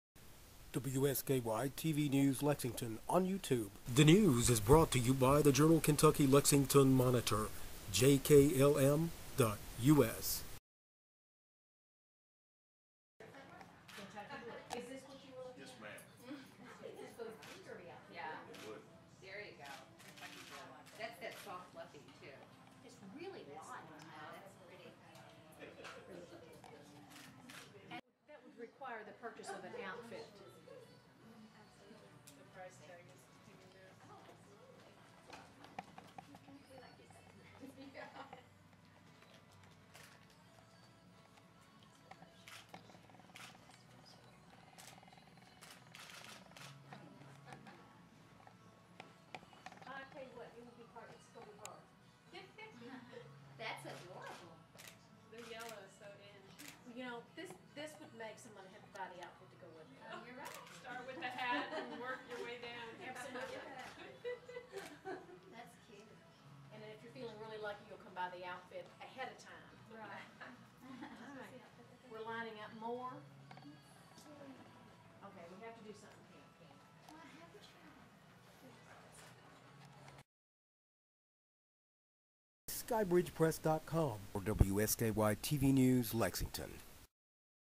First Lady Hunting for the Hottest Derby Hat Ever!
Part 3 of the series "First Lady Hunts a Derby Hat for Charity."

Which Derby hat will Kentucky First Lady, Jane Beshear, pick? The fun is watching how much fun First Lady Beshear has working the crowd.

She asked for feedback, listened to funny and serious fashion critiques from the news media, and kept c-o-o-l!

She will give the Derby hat to the Horses and Hope charity drawing held by Nitro of Frankfort, a ladies boutique with a big heart.

Google it and go there to register to win the Derby hat!

Our First Lady loves working for great causes for Kentucky like Horses and Hope which helps equine industry workers with health issues like cancer.

Go Google "Horses and Hope."